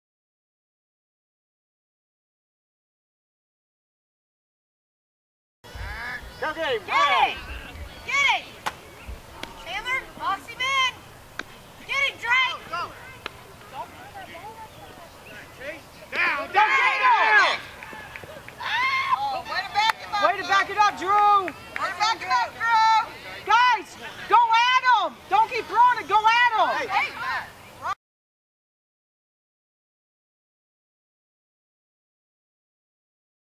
Box Him In - Chase Wymer
Canal Winchester Recreational League Baseball  Team #3 Baker.  This short video explains why you need to hustle, even if it appears for naught.  Video was  taken during the game against Pickerington on June 16, 2011. Chase is always going for the steal.  This time it paid off !!